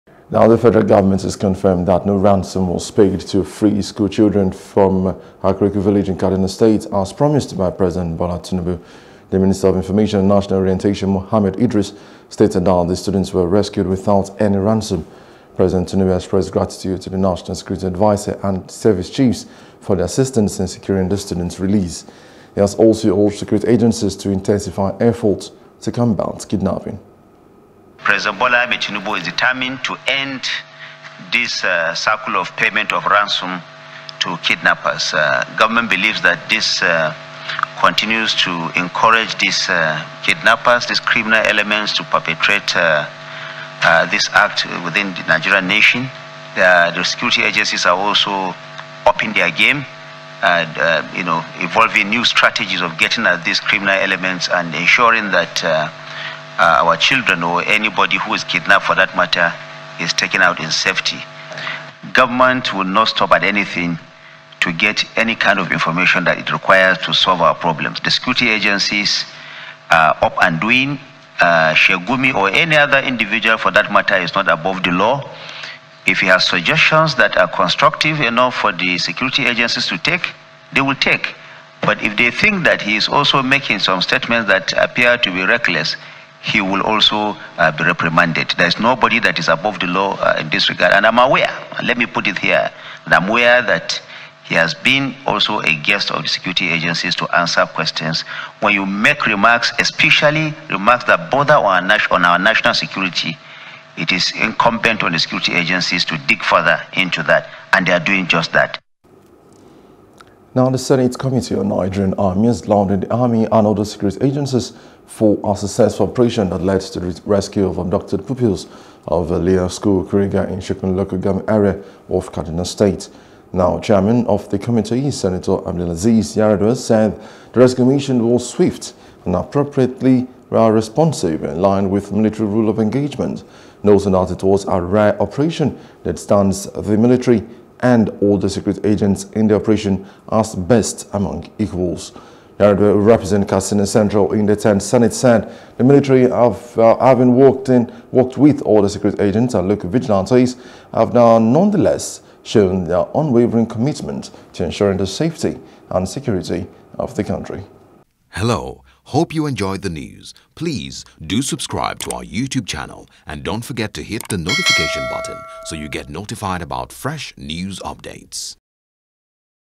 KADUNA ABDUCTION NO RANSOM PAID FOR KADUNA SCHOOLCHILDREN’S RELEASE — FEDERAL GOV’T
The Federal Government through the Minister of Information and National Orientation, Mohammed Idris has confirmed that no ransom was paid to free schoolchildren from Kuriga village in Kaduna State, as promised by President Bola Tinubu.